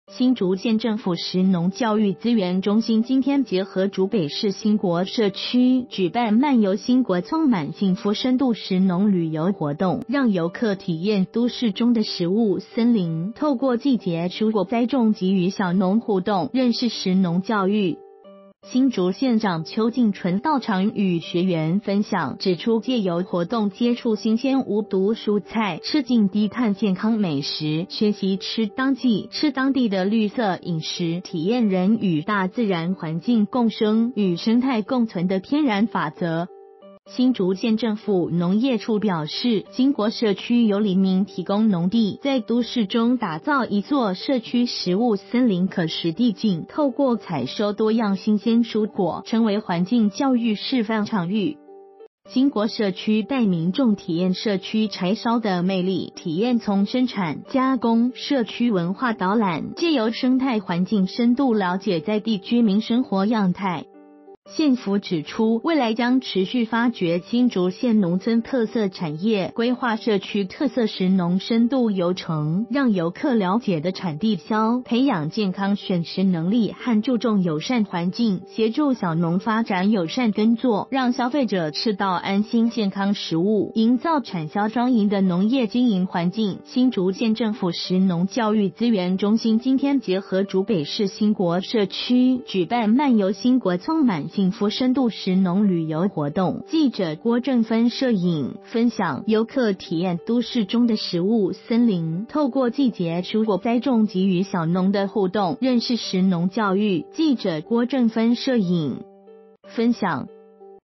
體驗農地到餐桌的旅程 竹縣新國社區推食農旅遊
發表評論評論如下。